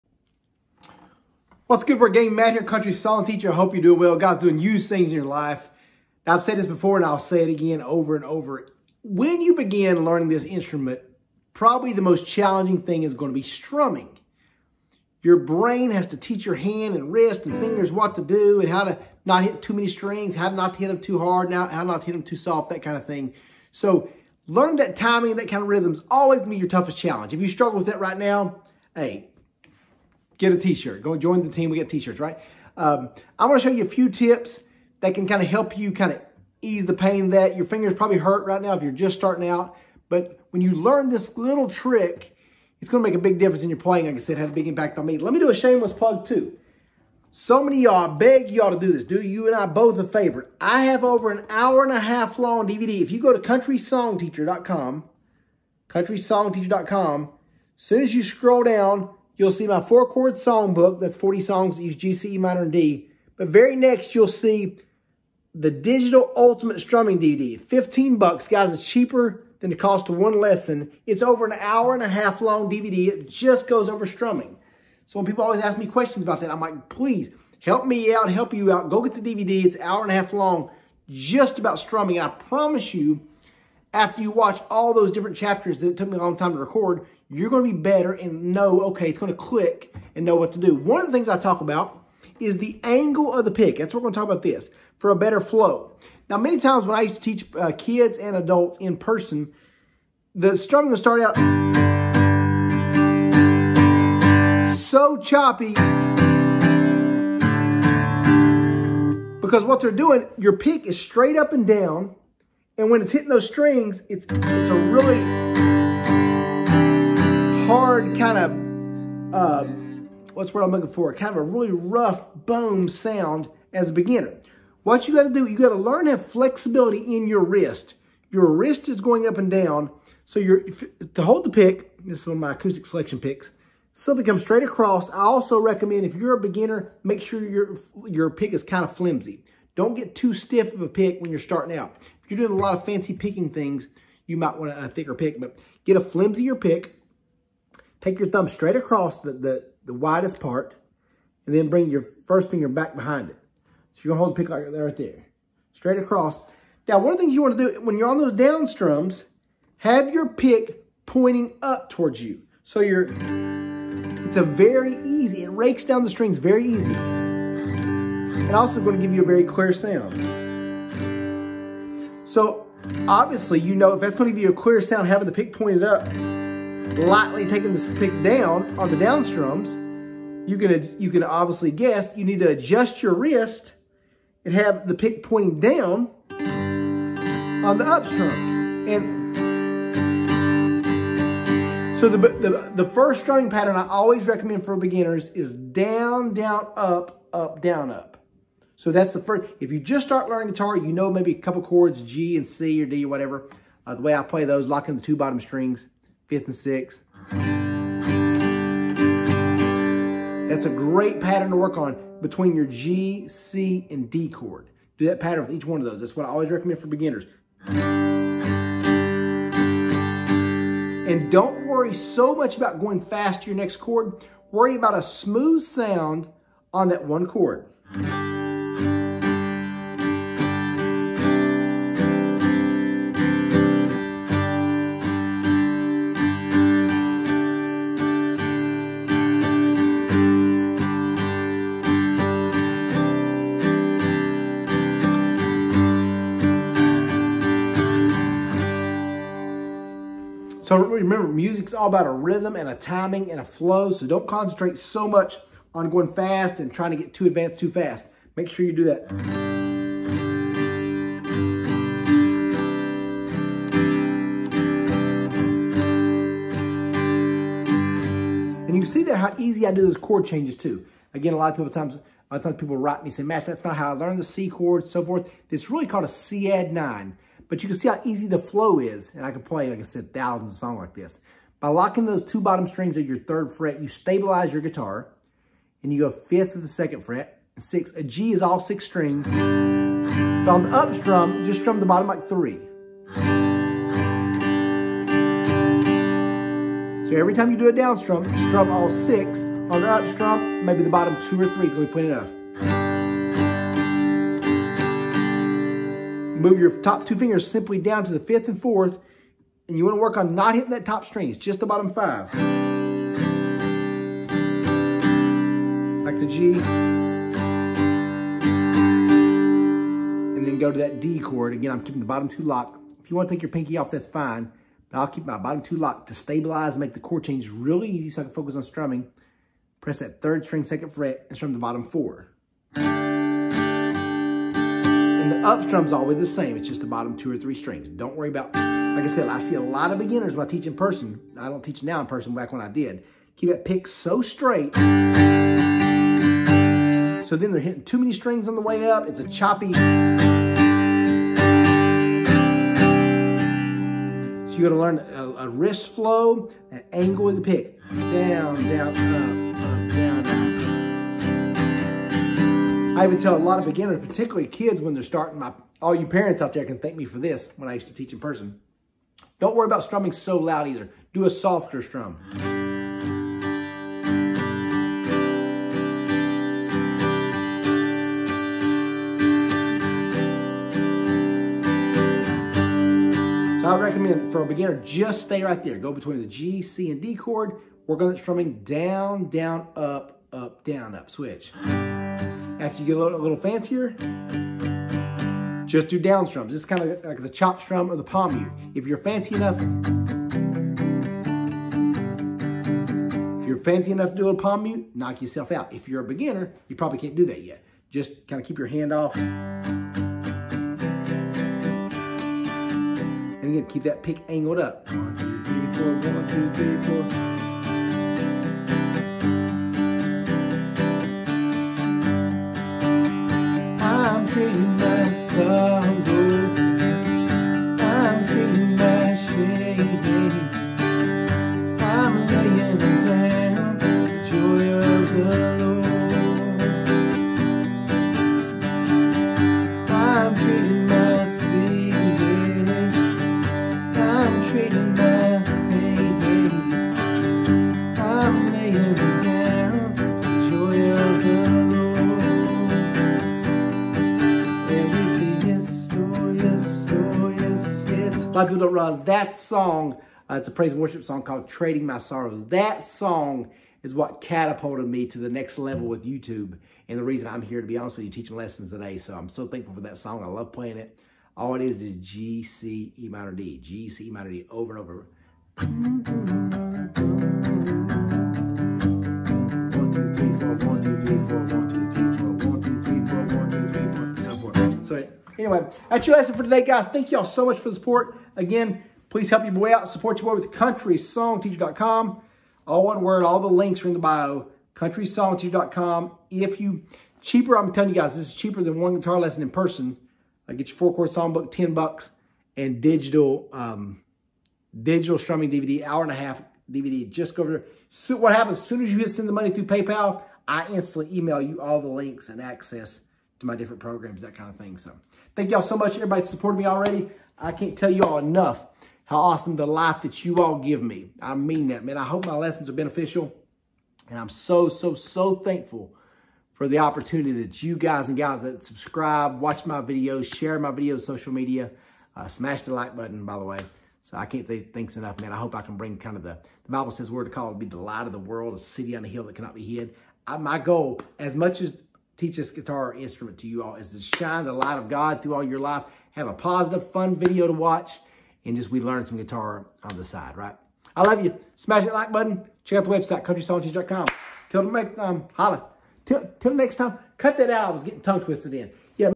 Angle Your Guitar Pick for Smoother Strumming | Beginner Tip
Try this simple trick to get a clearer, more defined sound on your strumming.